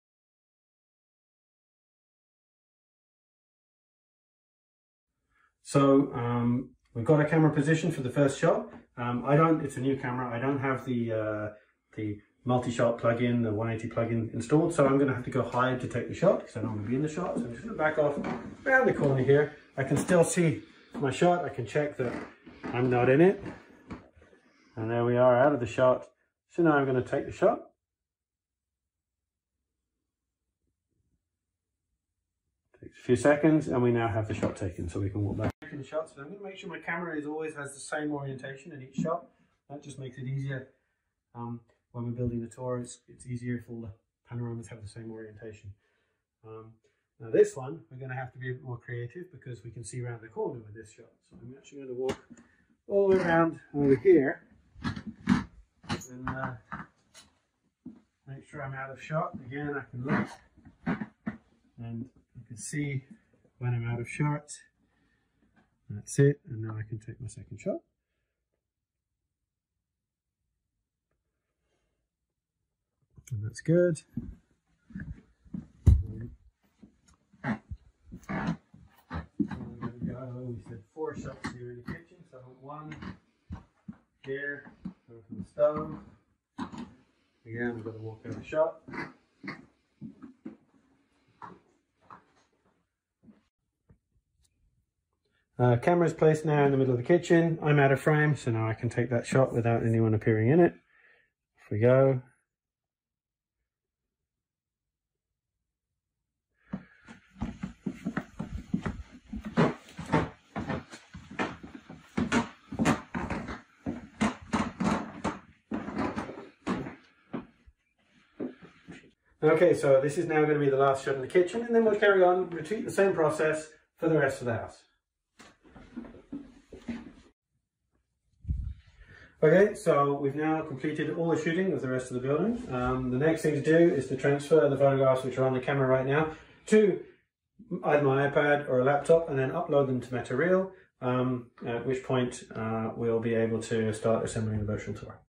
Metareal virtual tour shooting demo
Gareth walks you through an actual shoot for Metareal virtual tours.

Don't have a Metareal account yet?